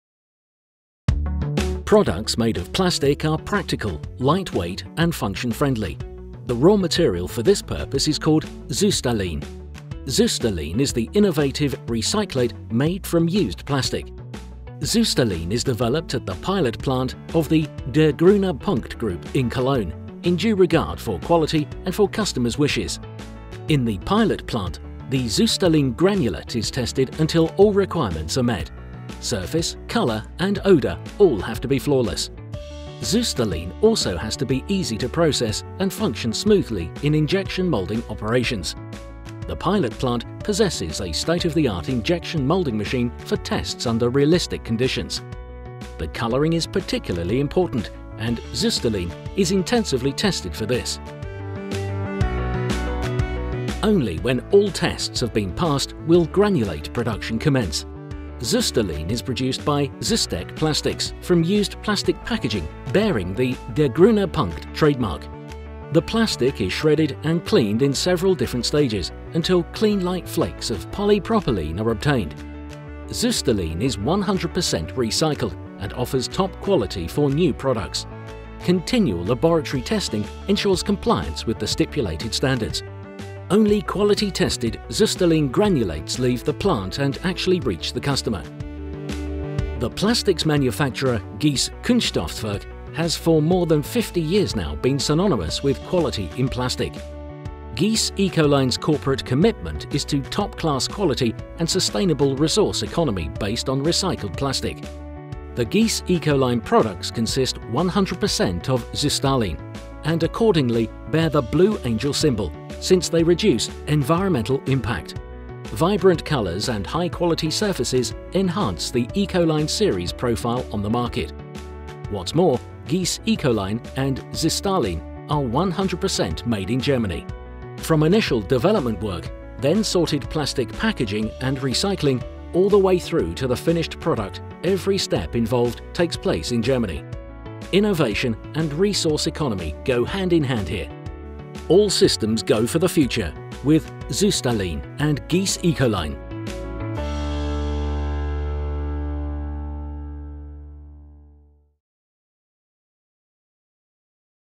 Raw material of the future - Production of the Ecoline series from Gies, made from Systalen
Systalen is the innovative recyclate from Der Grüne Punkt, made from used plastic. This video shows the production process used for making baskets in the Ecoline series of the Gies company. The Gies Ecoline products, which are made entirely from Systalen, bear the Blue Angel, since they are particularly resource-economical. Compared to other recycled products, they excel in terms of brilliant coloring and high-quality surfaces - they are almost indistinguishable from crates and baskets made from new plastics. Systalen is the plastic granulate of choice for sustainable products.